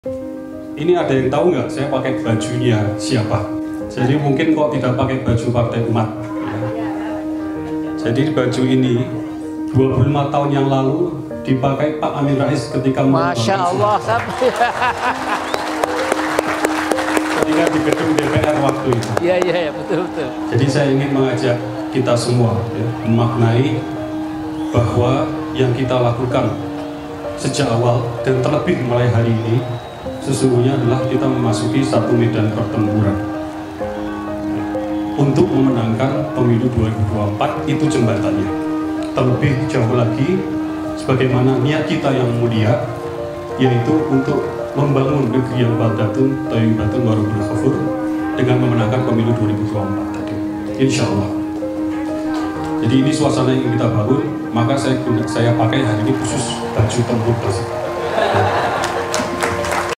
Semangat Reformasi '98 Harus Diteruskan!
Tahun 1998 ketika masa Reformasi, baju batik bersejarah ini dikenakan oleh M. Amien Rais ketika menurunkan Soeharto. Baju yang memiliki makna penuh perjuangan itu kini dikenakan oleh Ridho Rahmadi. Reformasi harus diteruskan!